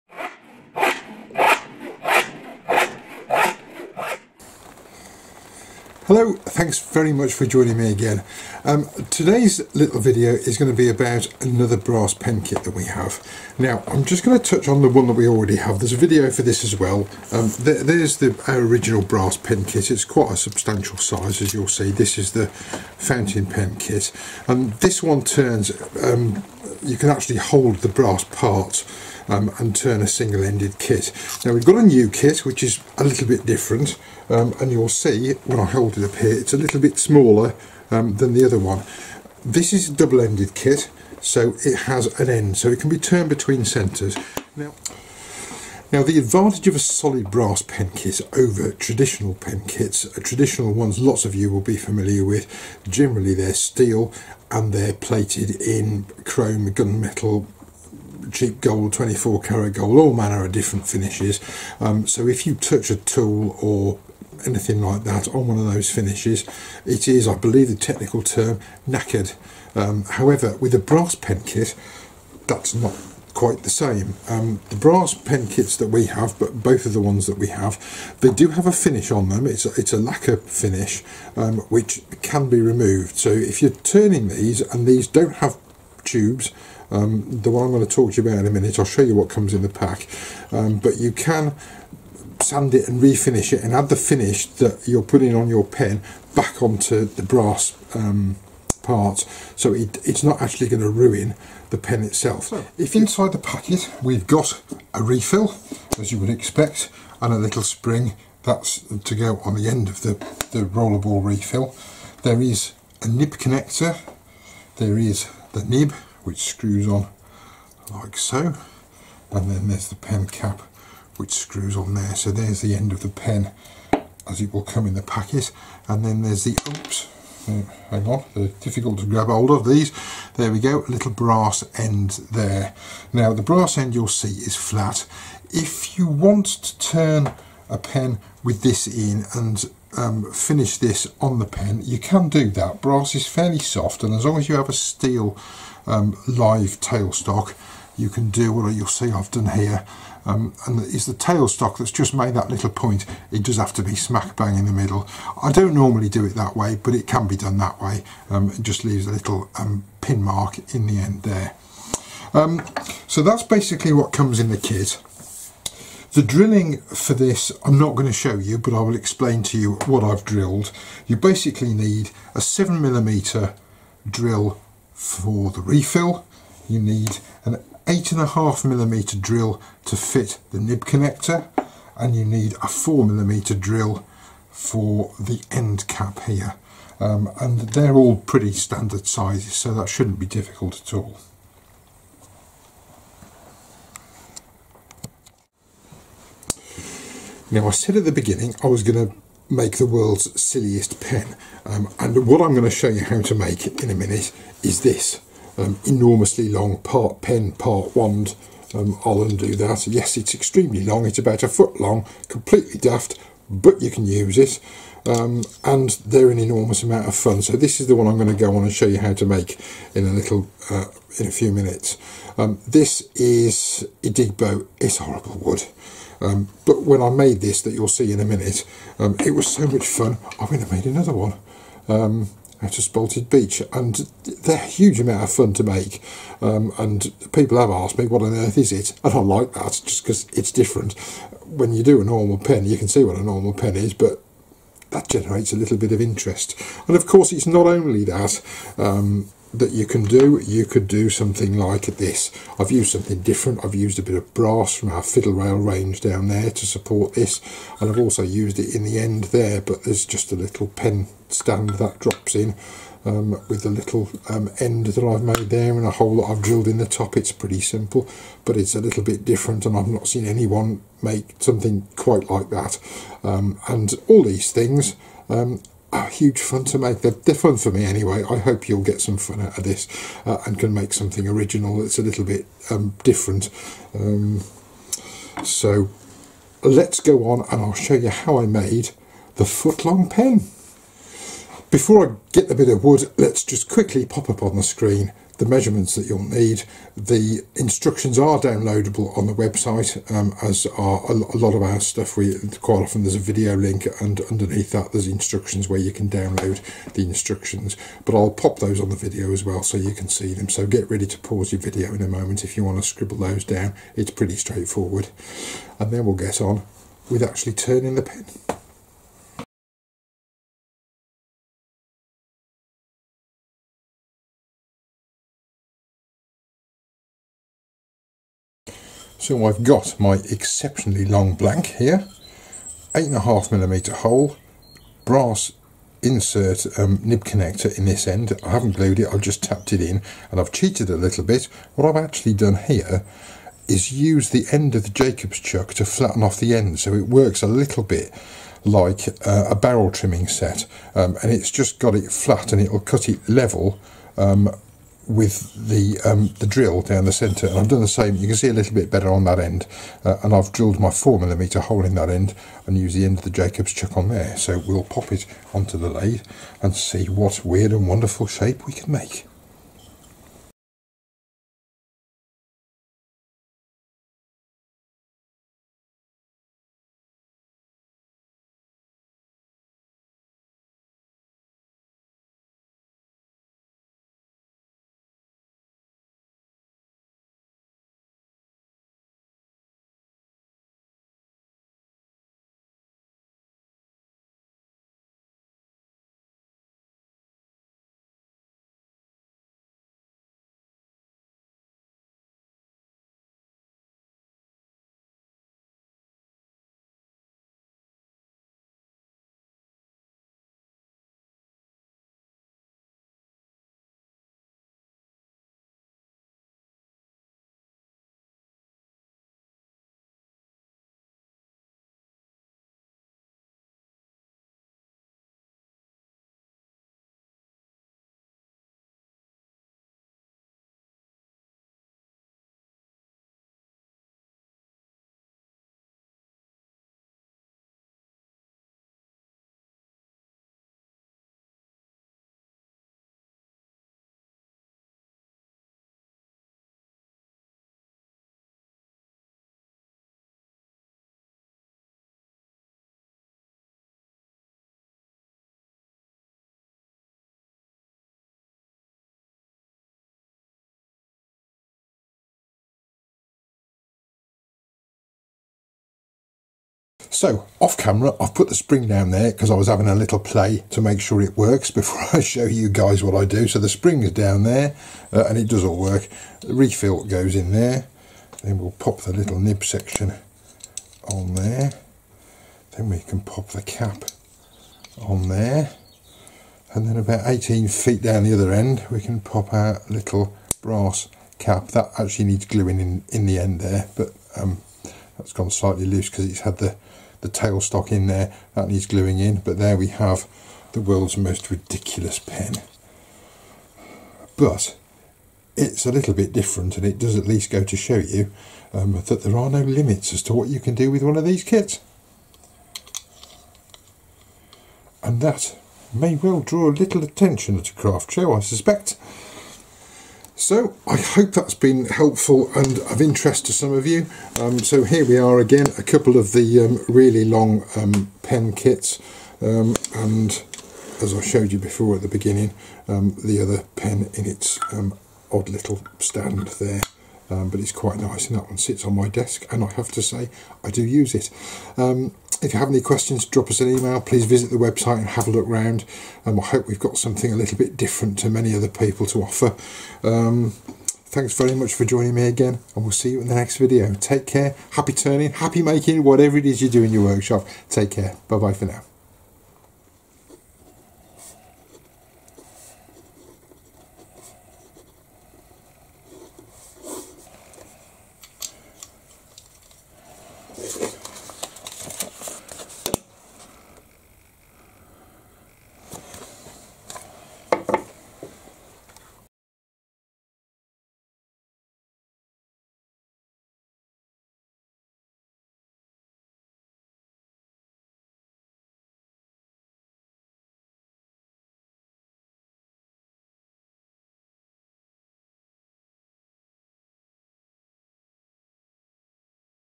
solid brass tubeless pen kit a VERY long pen
See me use the Prokraft SBTE solid brass tubeless pen kit to make a very long pen - just to show the versatility of the kit. Turned between centres this is about as much fun as you can have with a pen kit. No tubes, no bushings, no mandrel.